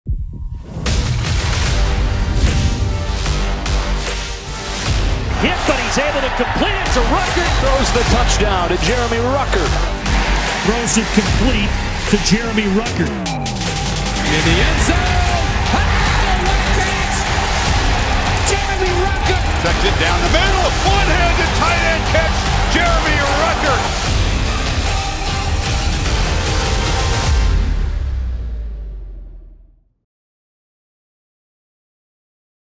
Jeremy Ruckert Highlights | The New York Jets | NFL
Check out highlights from the team's 101st overall draft pick, TE Jeremy Ruckert.